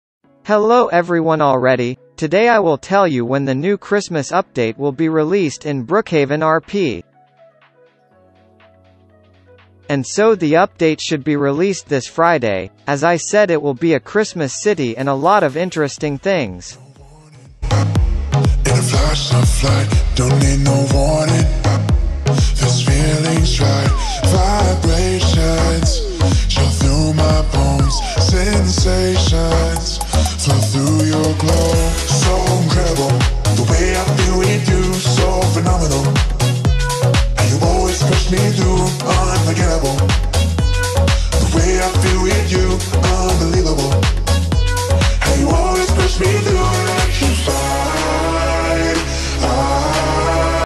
When is the UPDATE in Brookheaven RP ?? | Roblox news
Hello everyone, a new Christmas update is coming soon in your favorite roblox mode